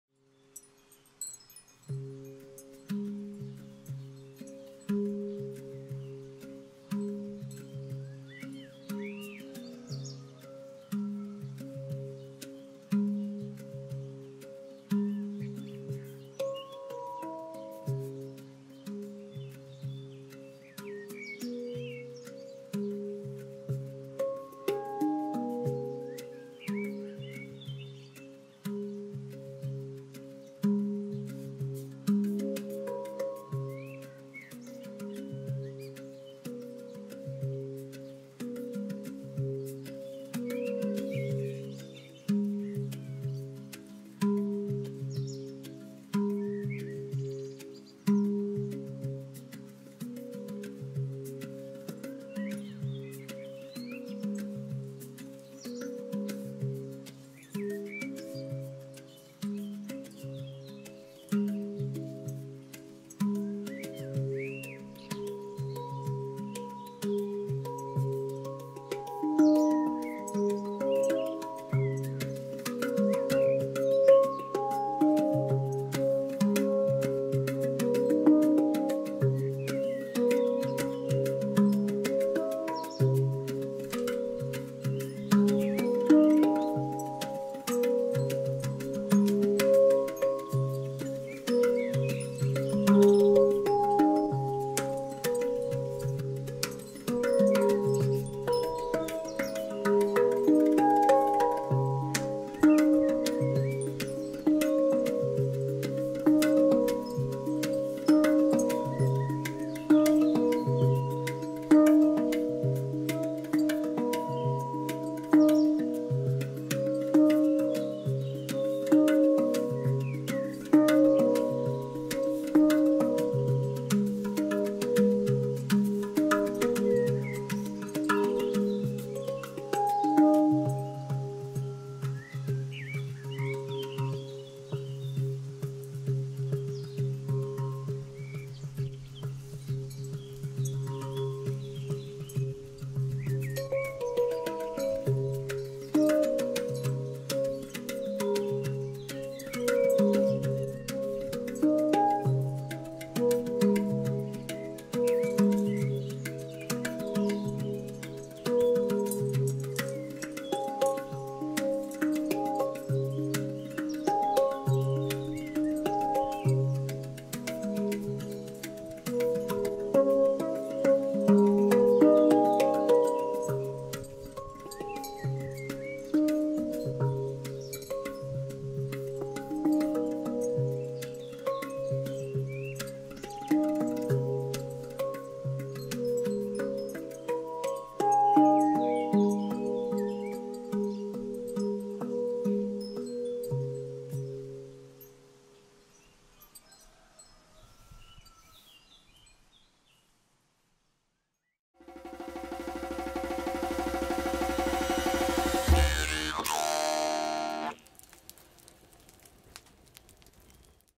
Handpan Shamanic Cis Ciska 432Hz by Terre
Handpan Music played by Thomas Tiefenbacher in an enchanting Nature enviroment. Listen to the Birds and frogs. All are giving a great concert.
Sound + Artist Thomas Tiefenbacher
Video Kondak Film